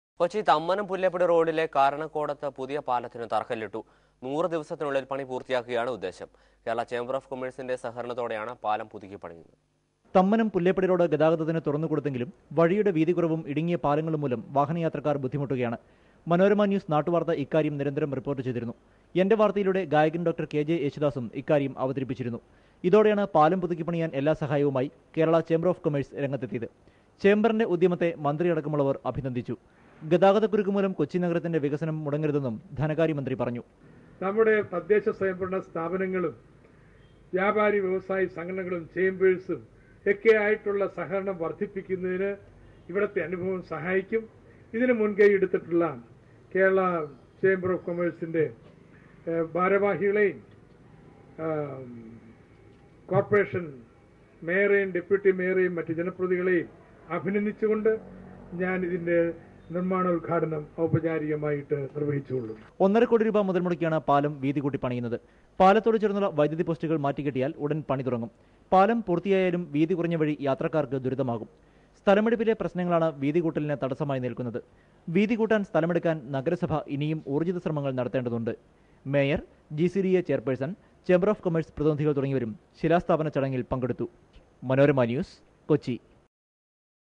Thammanam Pulleppady Road - KochiNow.com
Foundation stone laid for Karanakodam bridge on the four lane TP road going under construction soon.  The 22 metre wide bridge is constructed by KCCI will be completed in three months